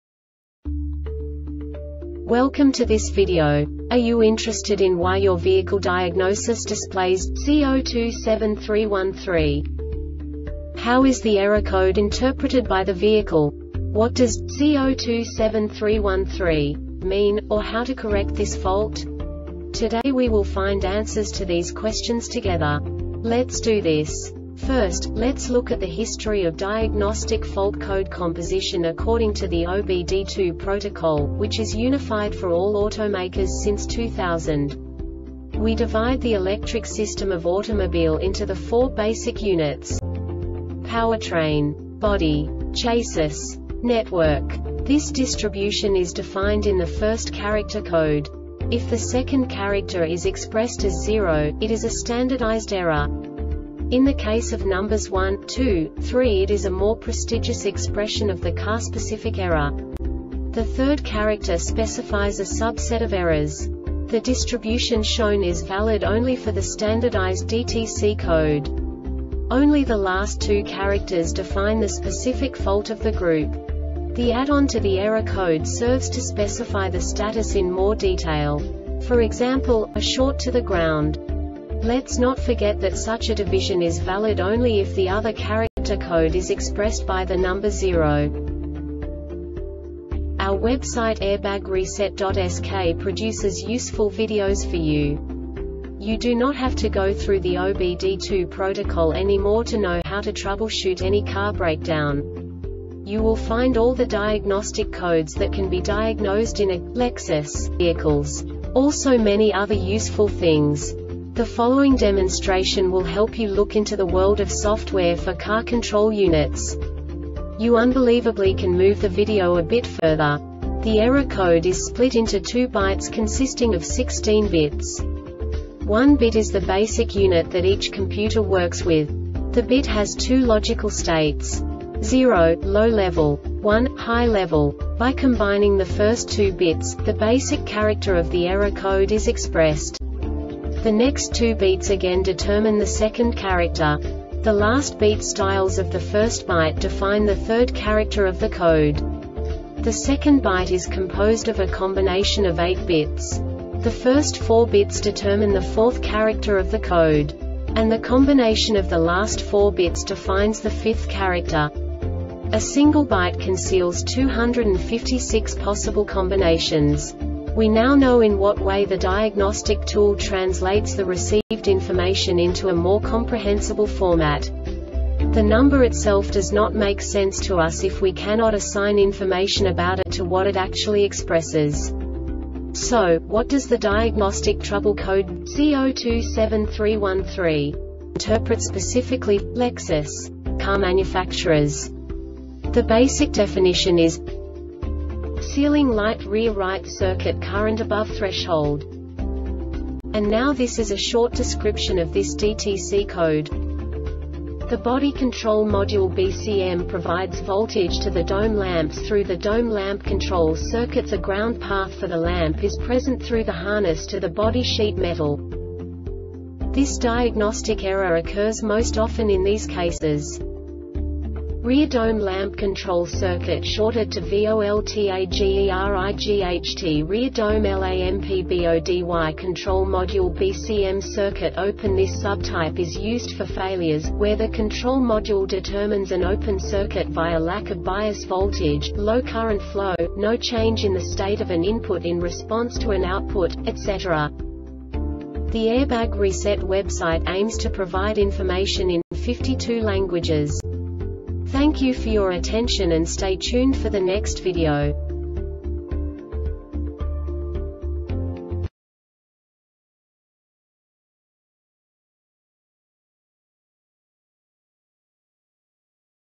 DTC Lexus C0273-13 Short Explanation
Contents:
0:21 Basic DTC analysis according to OBD2 protocol standard.
1:48 Insight into programming
2:58 A diagnostically understandable description of the Diagnostic Trouble Code
3:05 Basic error definition

Make:
Lexus
Code:
C0273 13
Definition:
Ceiling Light Rear Right-Circuit Current Above Threshold
Description:
The Body Control Module (BCM) provides voltage to the Dome Lamps through the Dome Lamp Control circuits. A ground path for the lamp is present through the harness to the body sheet metal.
Cause:
REAR DOME LAMP CONTROL CIRCUIT SHORTED TO VOLTAGERIGHT REAR DOME LAMPBODY CONTROL MODULE (BCM)
Failure Type:
Circuit Open